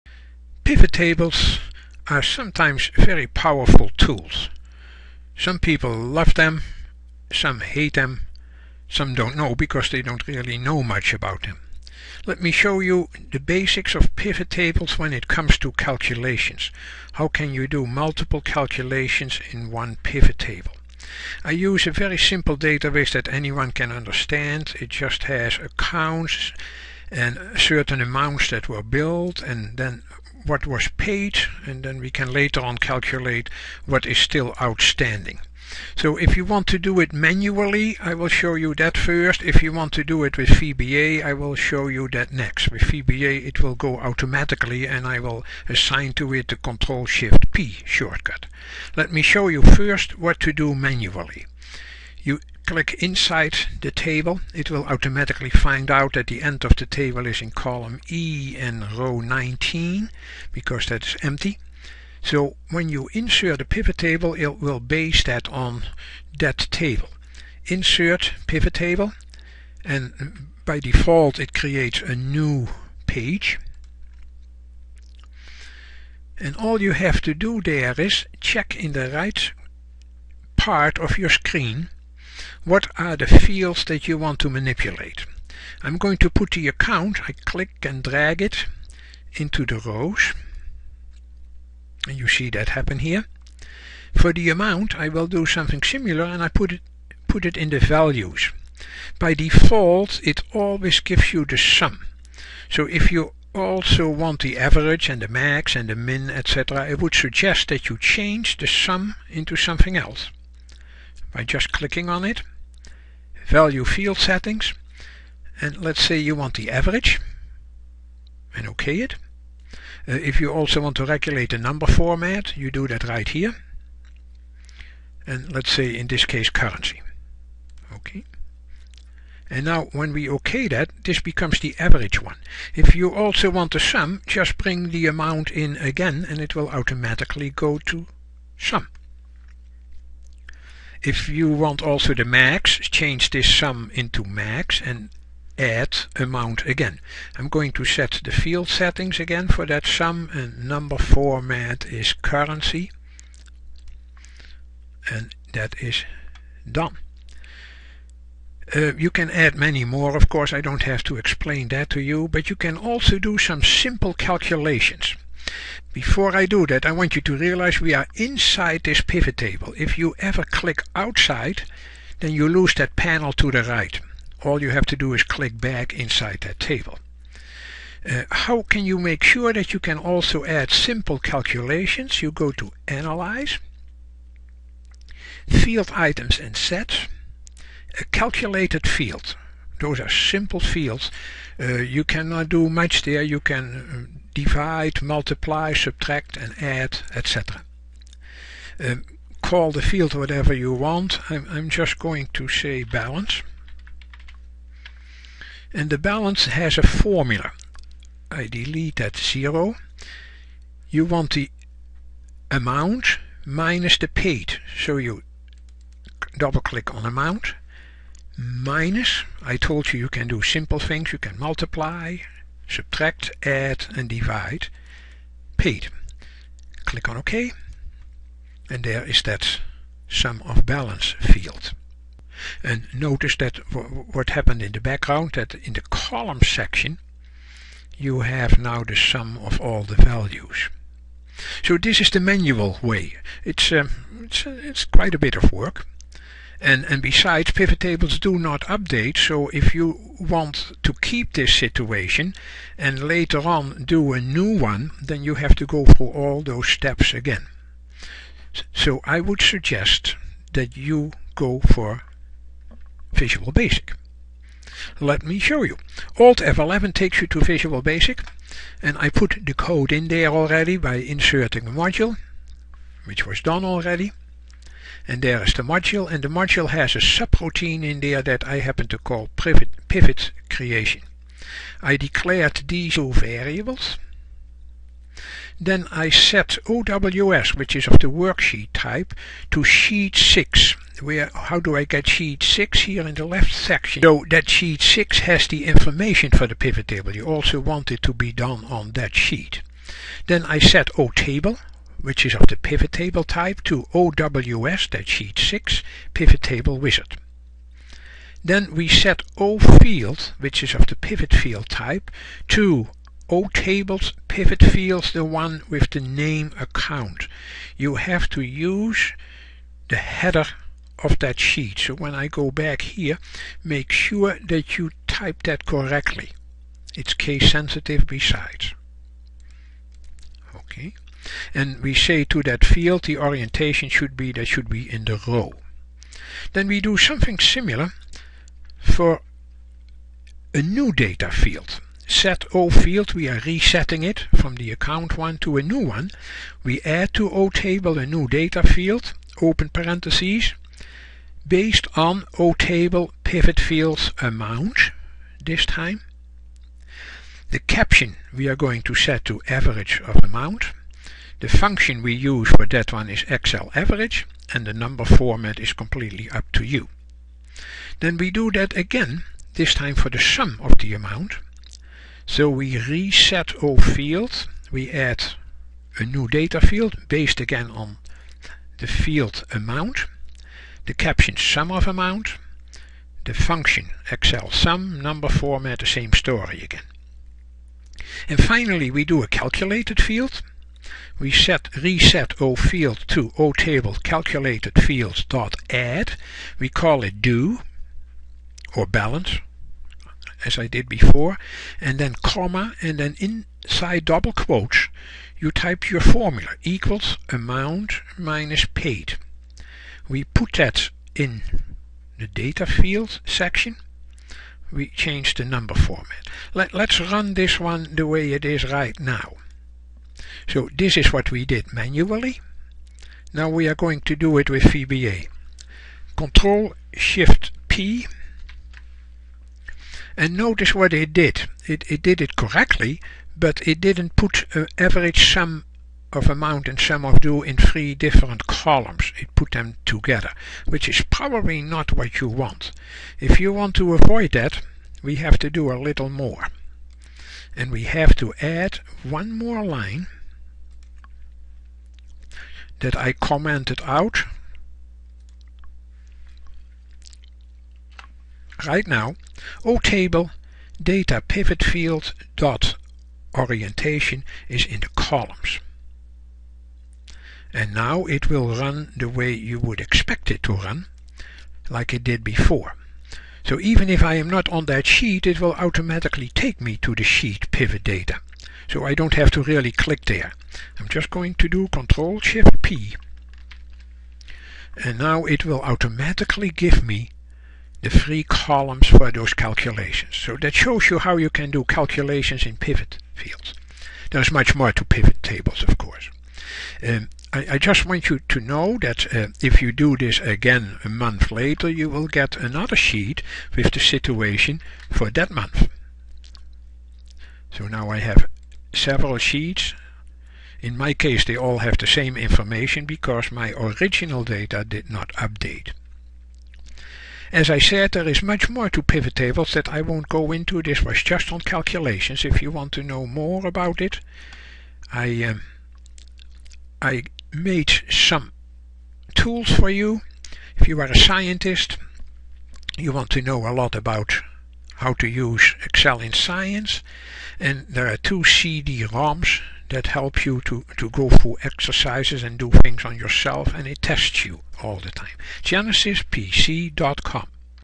Pivot Table Calculations: Manually or with VBA
Pivot Tables in Excel allow you to create a neat overview of your data with powerful calculated summaries. How do you achieve this manually? And how would you do this automatically with VBA? Let's find out.